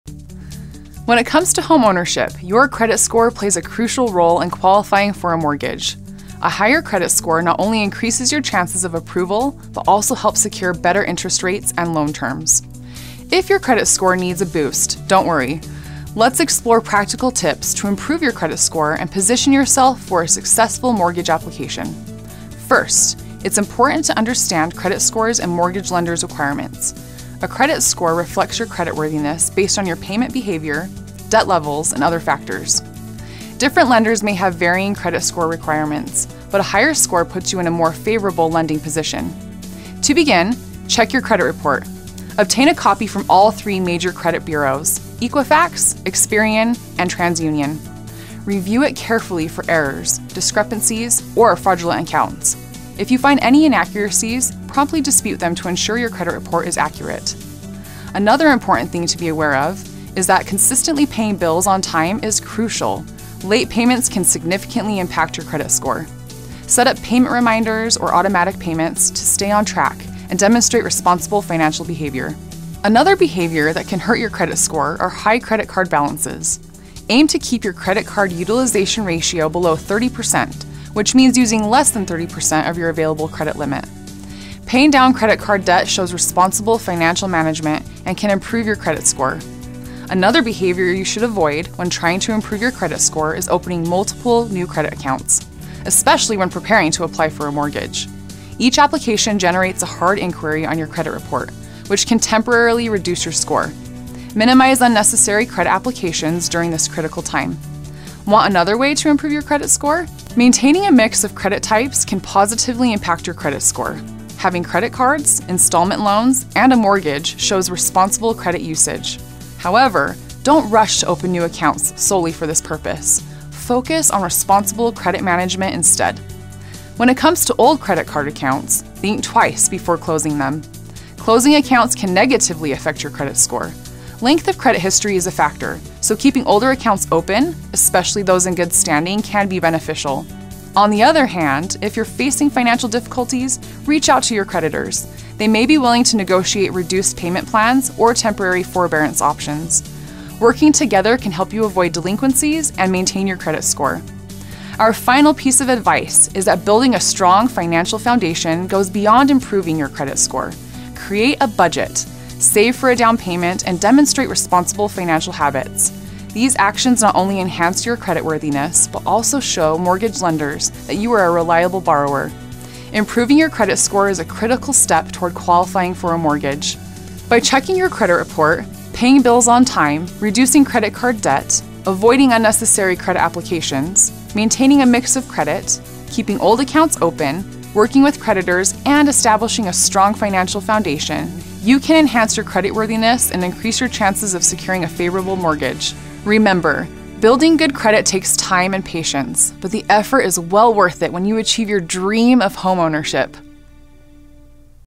How To Improve Your Credit Score And Qualify For A Mortgage
VA Home Loan: LEARN: EP. 71- How To Improve Your Credit Score & Qualify For A Mortgage

Summary:
🏡 Boosting Your Credit Score for a Dream Home 🚀 Learn how to improve your credit score for a successful mortgage application! Understand credit scores, review your credit report, pay bills on time, manage credit card balances, avoid opening new accounts, maintain a credit mix, and keep old accounts open. Don't forget to build a strong financial foundation with budgeting, saving, and responsible habits.

🤔🤔🤔🤔Questions & Answers🤓🤓🤓🤓
🤔Question: What role does a higher credit score play in mortgage applications?
🤓Answer: A higher credit score increases your chances of mortgage approval and helps secure better interest rates and loan terms.

🤔Question: How can I check and correct errors on my credit report?
🤓Answer: Obtain your credit report from major bureaus, like Equifax, Experian, and TransUnion. Review it for errors, discrepancies, or fraudulent accounts, and promptly dispute inaccuracies.

🤔Question: Why is maintaining a credit card utilization ratio important?
🤓Keeping your credit card utilization below 30% of your limit demonstrates responsible financial management and positively impacts your credit score.

🤔Question: Should I close old credit card accounts?
🤓Answer: While closing accounts can negatively affect your credit score, keeping older accounts open, especially those in good standing, benefits your credit history. Consider carefully before closing them.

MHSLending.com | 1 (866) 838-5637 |
Equal Opportunity Lender
MortgageOne, Inc NMLS #898812